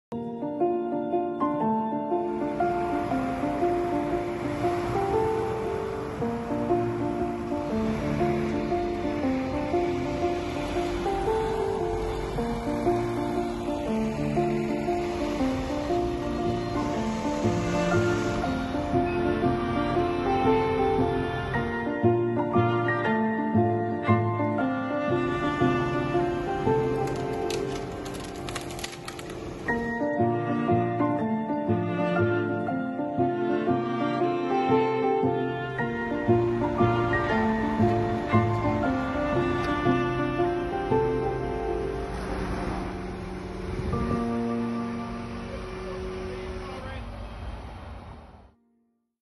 Roots complete mush big tree
Video from Clive Cummings
Another option we do for unsafe trees. Note the rotted roots base of tree lifted straight out of the ground. Location The Avenue Lower Sunbury.